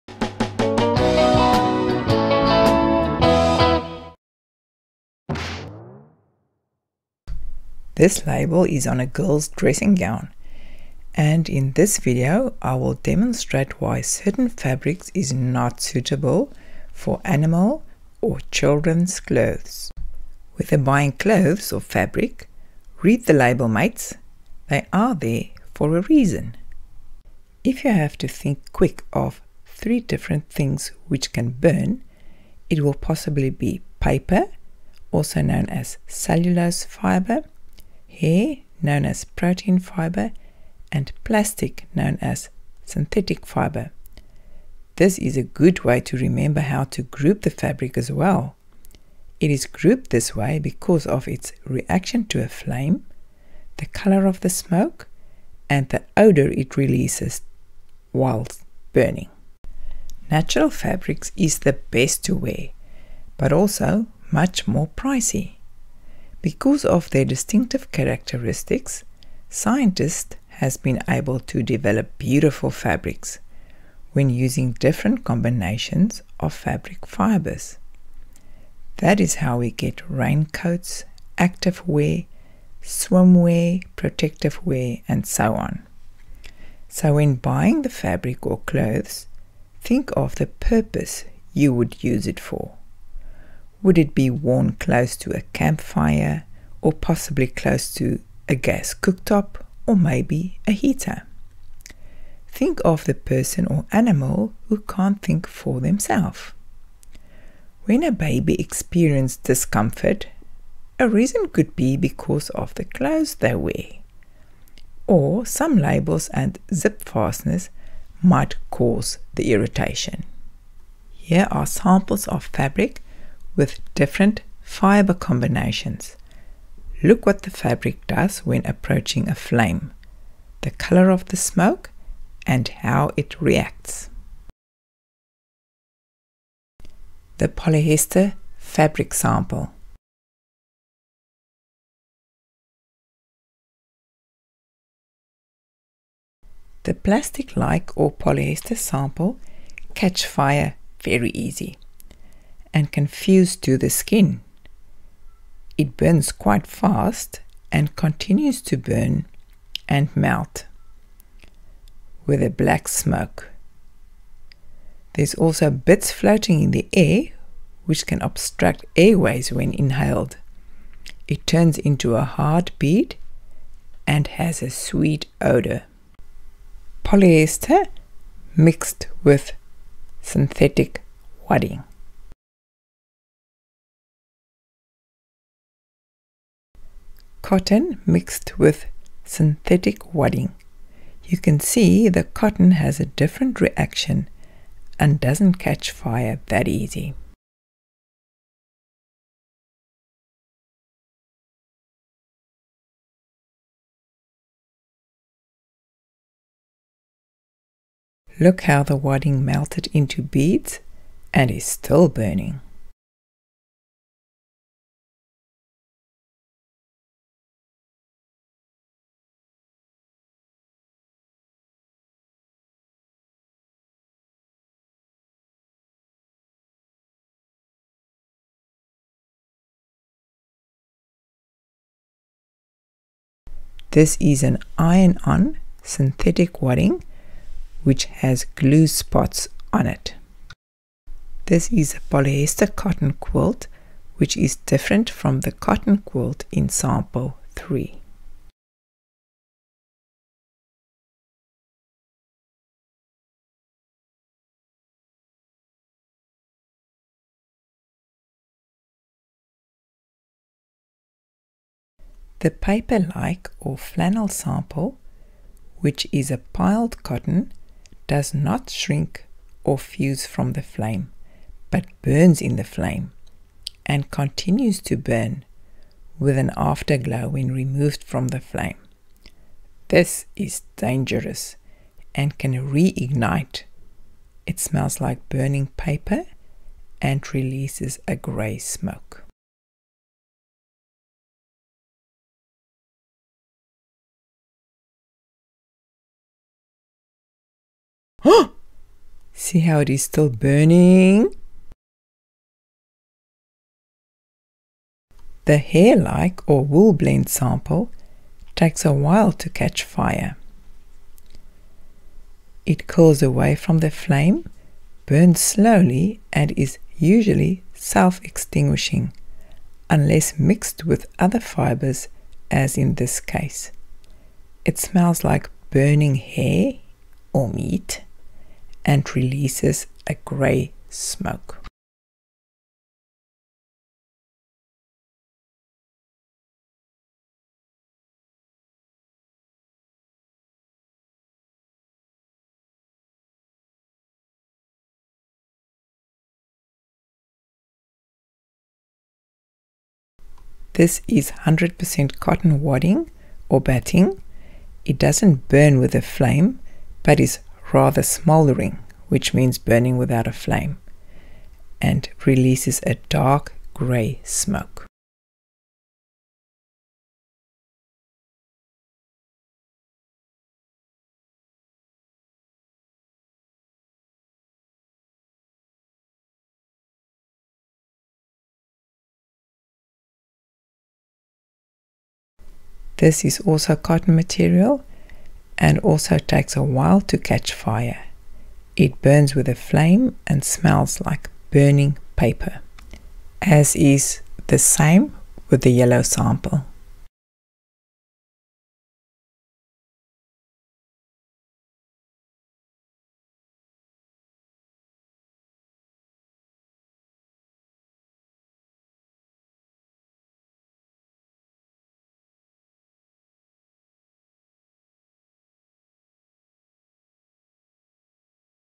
How to choose suitable fabric or material for a particular sewing project
This is a demonstration of how different fabrics reacts to a burning flame